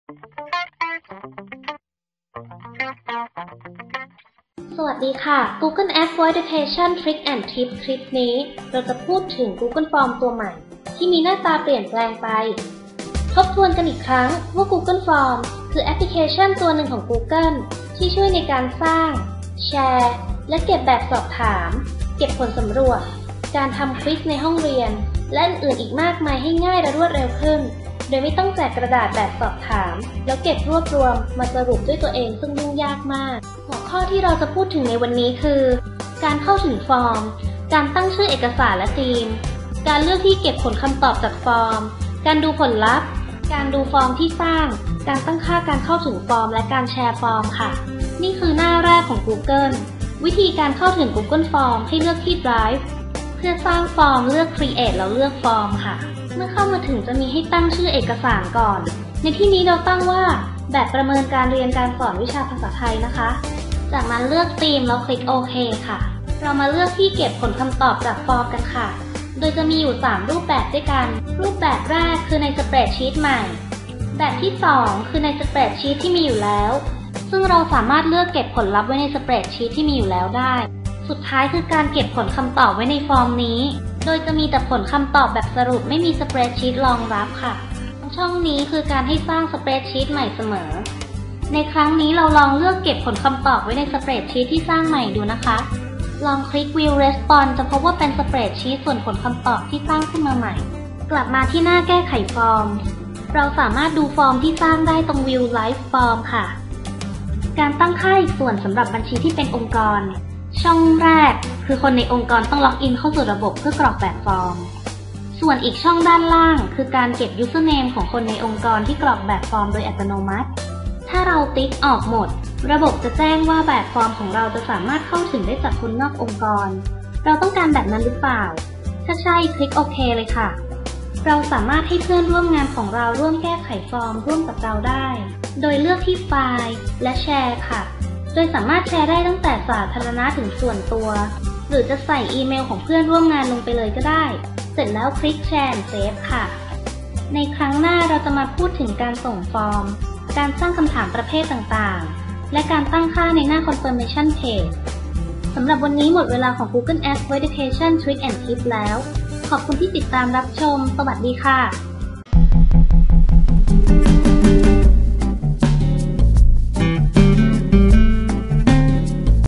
Google Apps for education tricks & tips - new Google form (1/2)
This video is create for Google Apps for Education training by CRM-Charity Foundation
Watch in HD(720 p) is better!!